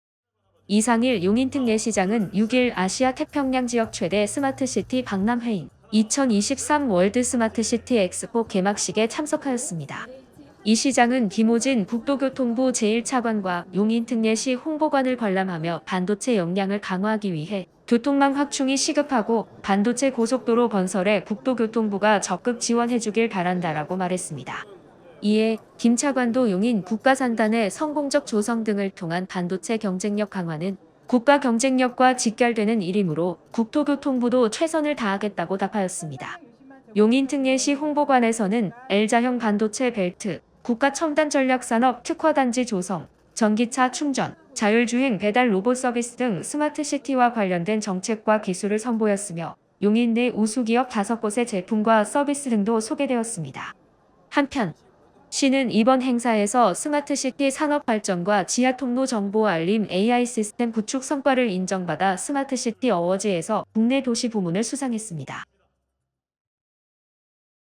[용인시정뉴스] 이상일 용인특례시장, ‘2023 월드 스마트 시티 엑스포 ’참석
이상일 용인특례시장은 6일
 아시아 태평양 지역 최대 스마트시티 박람회인
‘2023 월드 스마트시티 엑스포 개막식’에 참석하였습니다.

 이 시장은 김오진 국토교통부 제1차관과
 용인특례시 홍보관을 관람하며
‘반도체 역량을 강화하기 위해 교통망 확충이 시급하고,
 반도체 고속도로 건설에
 국토교통부가 적극 지원해주길 바란다’ 라고 말했습니다.
 이에, 김 차관도‘용인 국가산단의 성공적 조성 등을 통한 반도체 경쟁력 강화는 국가경쟁력과 직결되는 일이므로
 국토교통부도 최선을 다하겠다고’ 답하였습니다.

 용인특례시 홍보관에서는
 L자형 반도체 벨트, 국가첨단전략산업 특화단지 조성,
 전기차 충전, 자율주행 배달 로봇 서비스 등
 스마트시티와 관련된 정책과 기술을 선보였으며,
 용인 내 우수 기업 5곳의 제품과 서비스 등도
 소개되었습니다.

 한편, 시는 이번 행사에서 스마트시티 산업 발전과
 ‘지하통로 정보 알림 AI시스템 구축’성과를 인정받아
 스마트시티 어워즈에서‘국내 도시’ 부문을 수상했습니다.